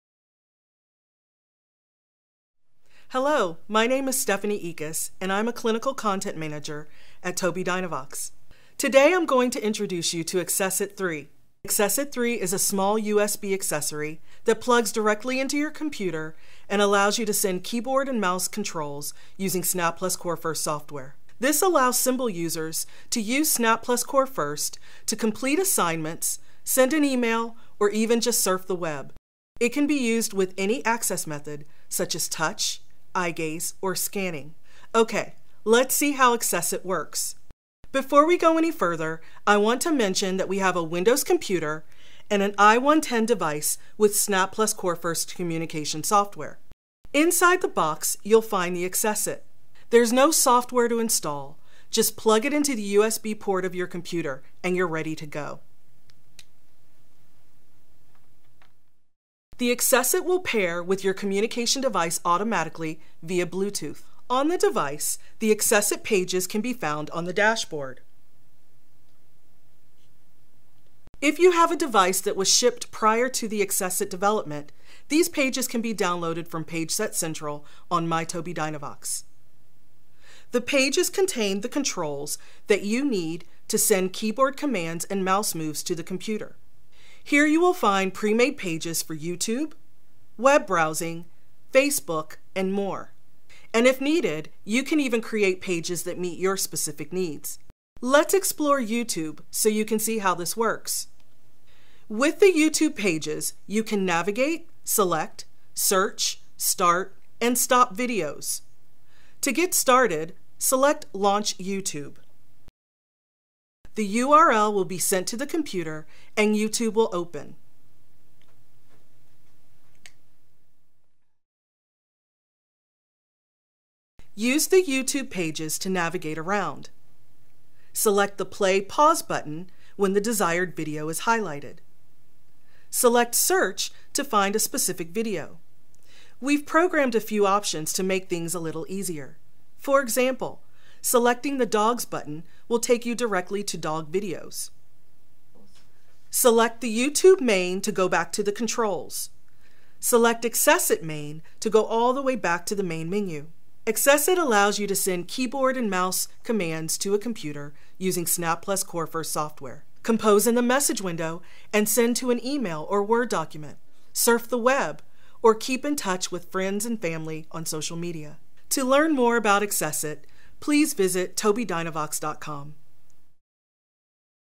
Introduction to AccessIT® 3
AccessIT 3 is a small USB accessory that plugs into a Windows computer and gives you complete control via Snap + Core First software. Watch as Stephanie, a Clinical Content Manager at Tobii Dynavox, introduces you to the product and demonstrates how to access YouTube with it.